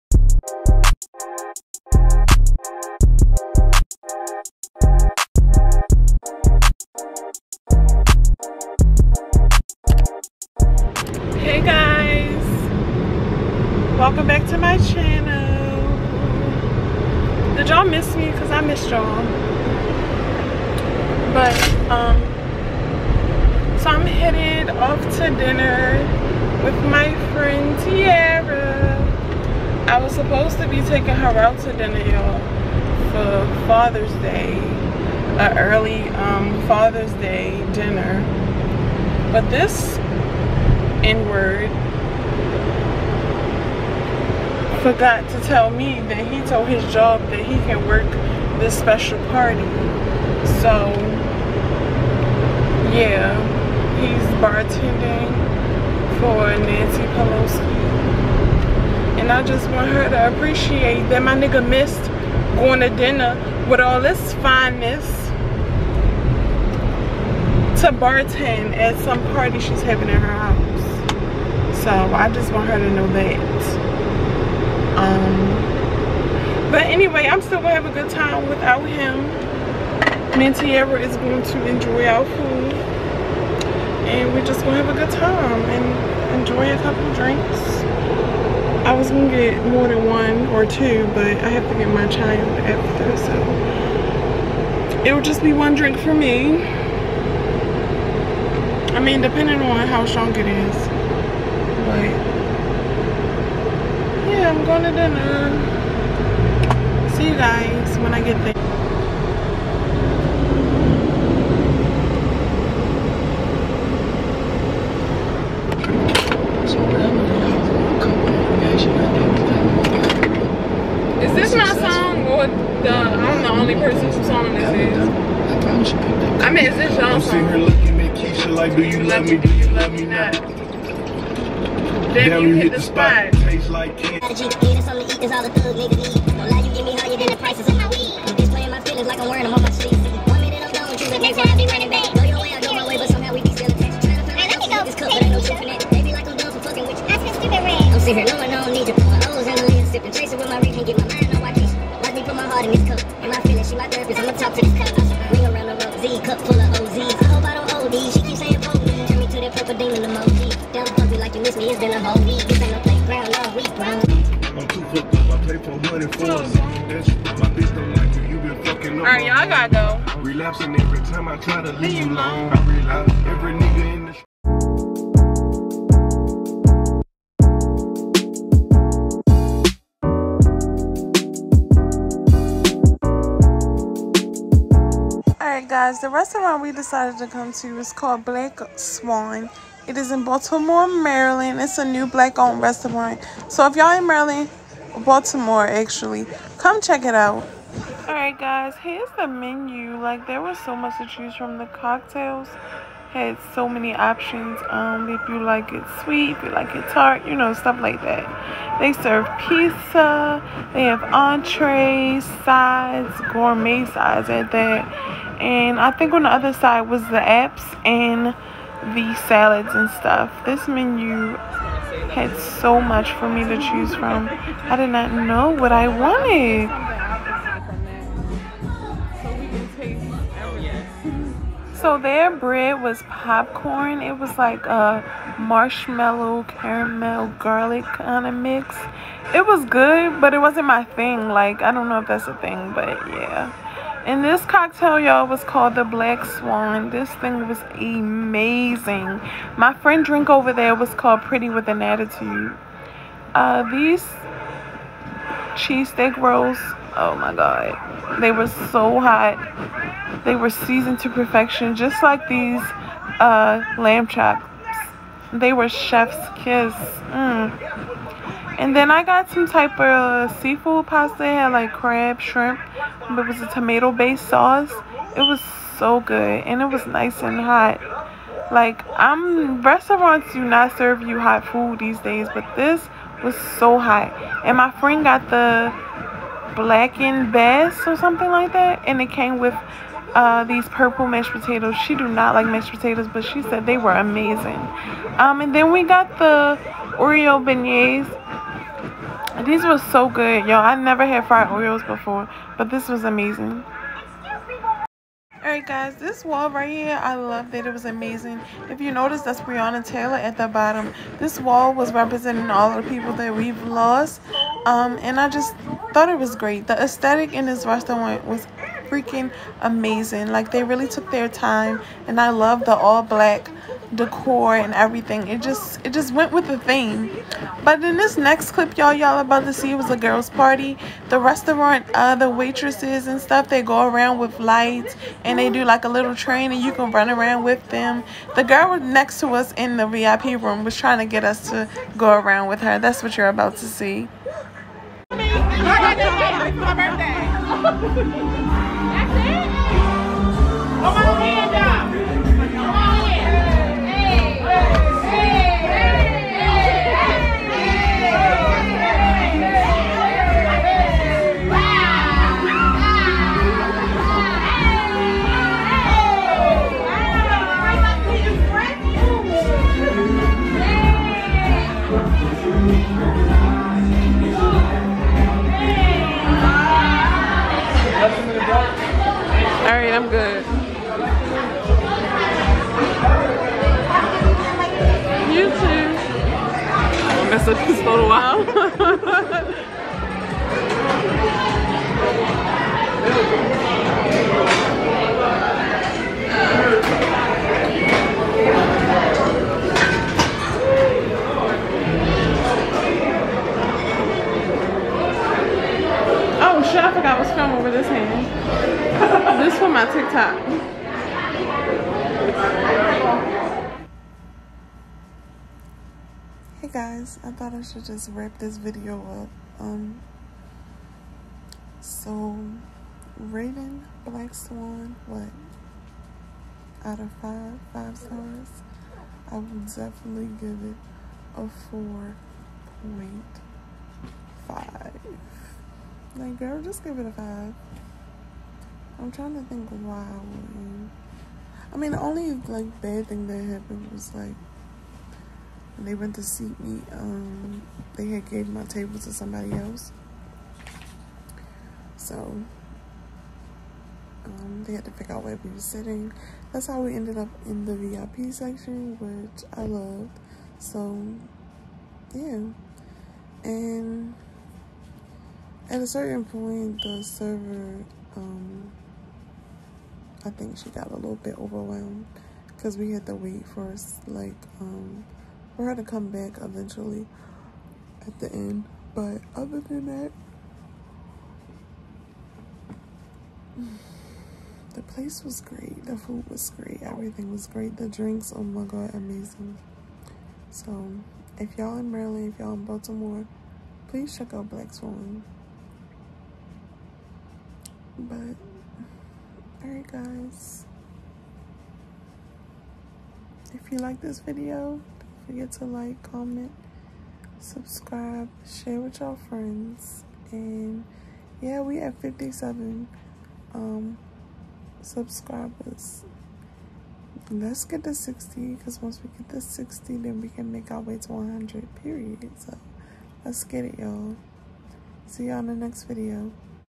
Vlog| Blk Swan Restaurant Review: Date your friends
Hey AB Babies Hope you enjoy this video !

If you’re not supporting black businesses then what are you doing?
I look forward to supporting more black businesses this summer, so comment some recommendations.

Thanks for always watching ! And if you are new here, what you waiting on?

Much love